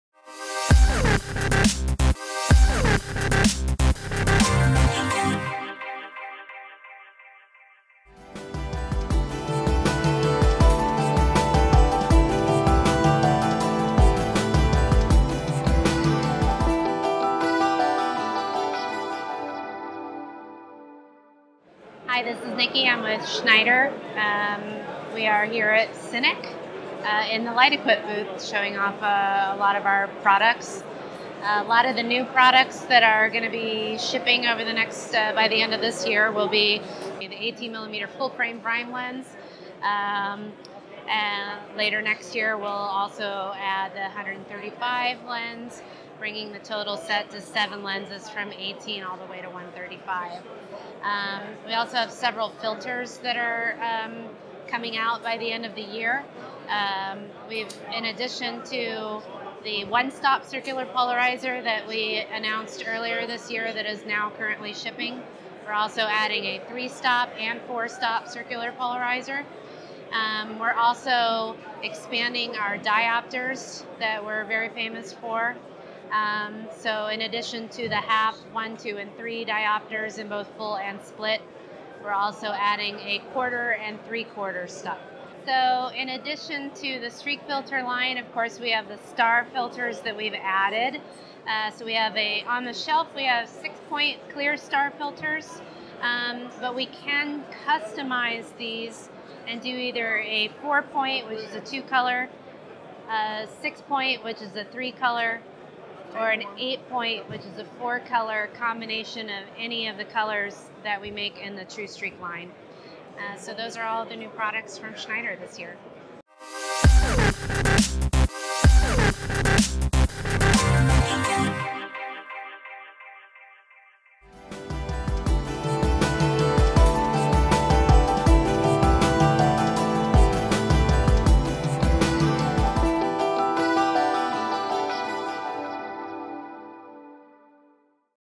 CINEC 2016 - SCHNEIDER OPTICS - NIKI MUSTAIN - VIDEO NEWS BY STEFANO SPITI AIC IMAGO
CINEC 2016 VIDEO-NEWS BY STEFANO SPITI AIC IMAGO AT SCHNEIDER OPTICS BOOTH WITH NIKI MUSTAIN -SALES